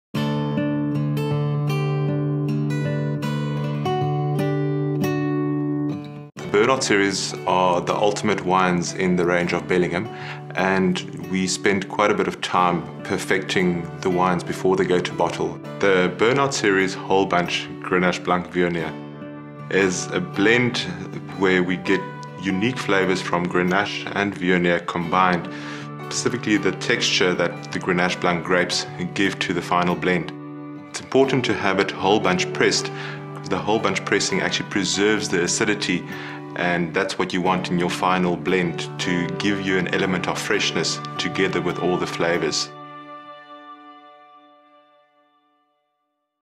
Grenache Blanc Voignier
The grapes used to make this wine are hand-picked and hand-selected, nurtured every step of the way, in true Bernard dedication. Whole bunch pressing gently extracts the juice which after fermentation is allowed to mature in French oak barrels for a period of 6 months. A unique and intriguing wine with Viognier characteristics of pear, ripe golden apples and underlying floral fragrances beautifully complemented by mandarine orange aromas and a touch of peach blossom from Grenache Blanc.